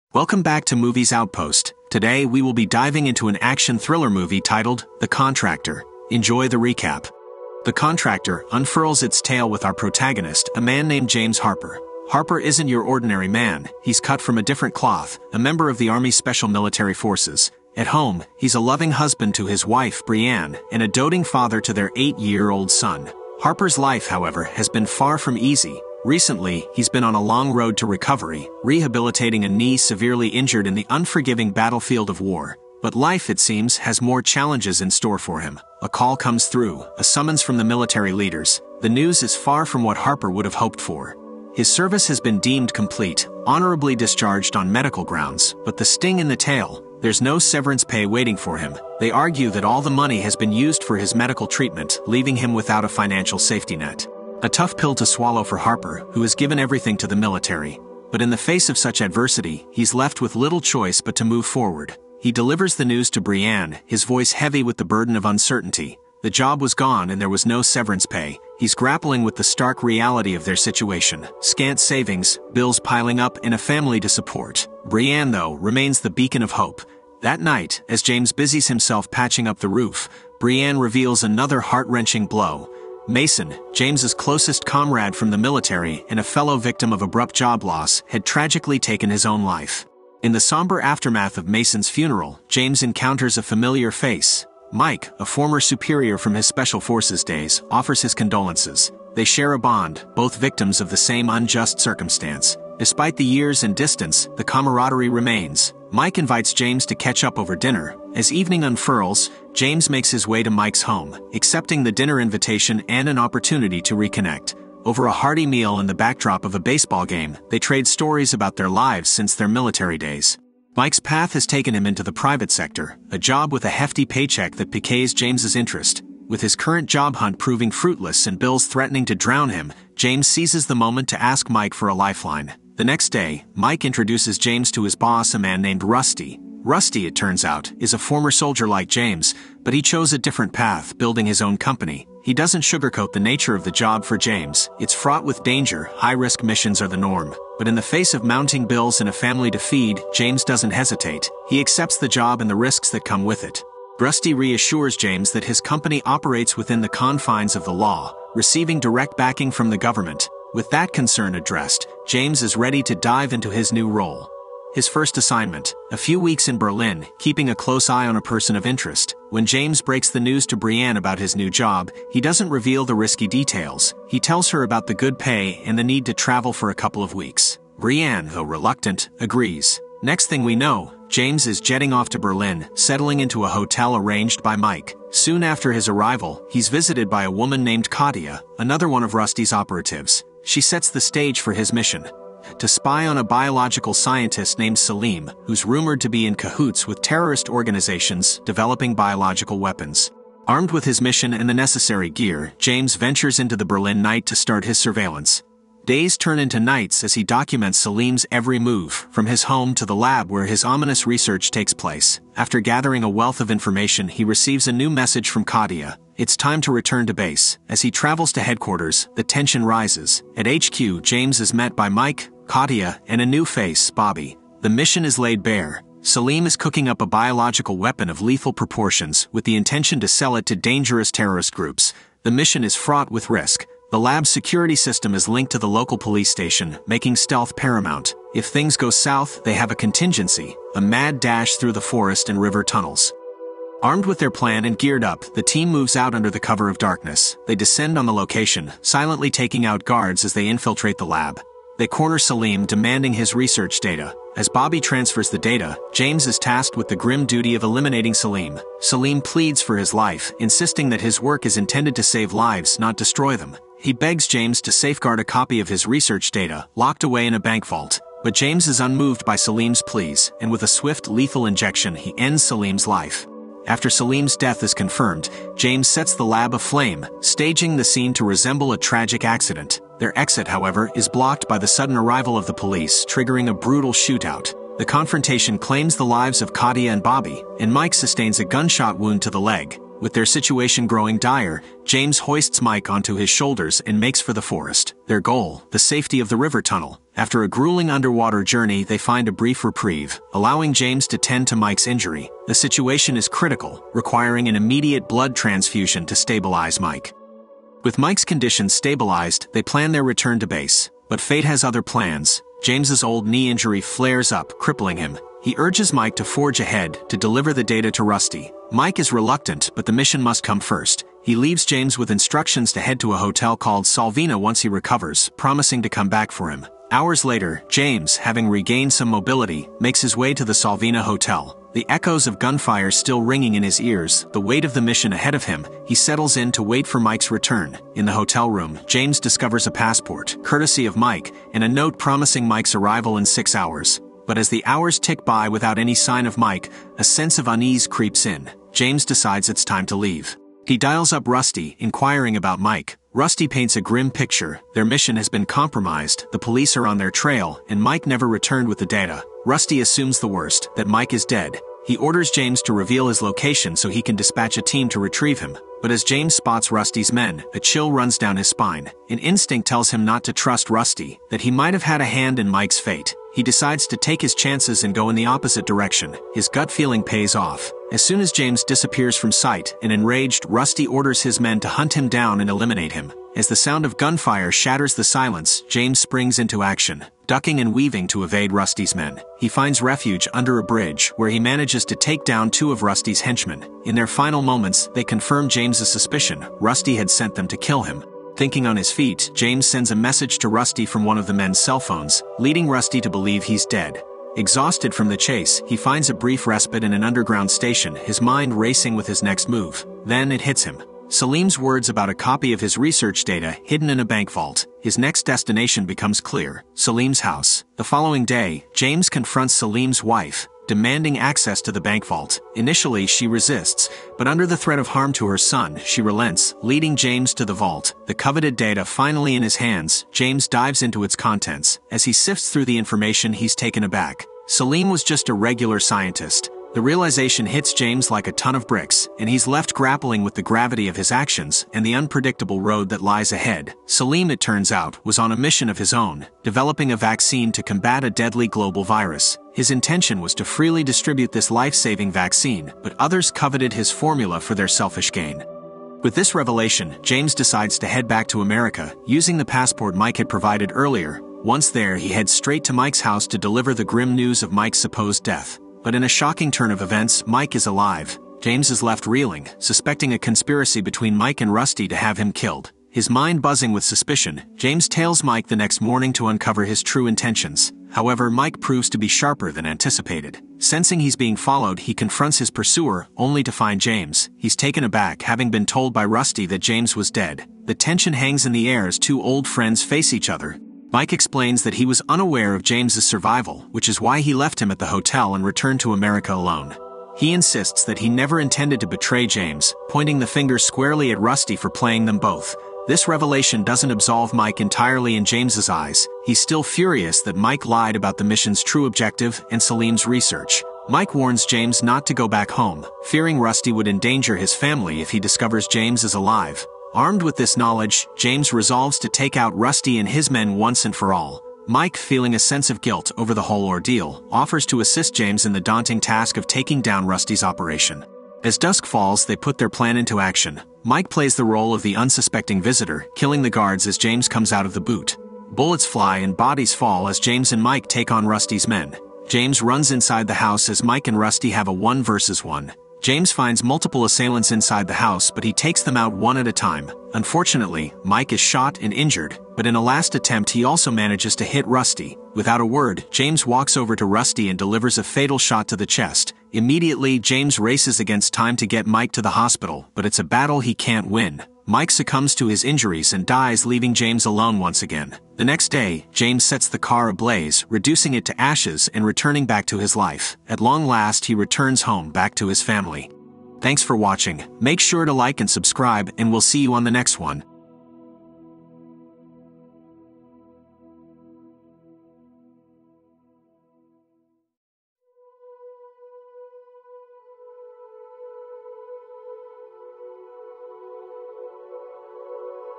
EX-SOLDIER JOINS MERCENARY GROUP BUT THEY DOUBLE-CROSS HIM
A discharged U.S. Special Forces sergeant, James Harper, risks everything for his family when he joins a private contracting organization.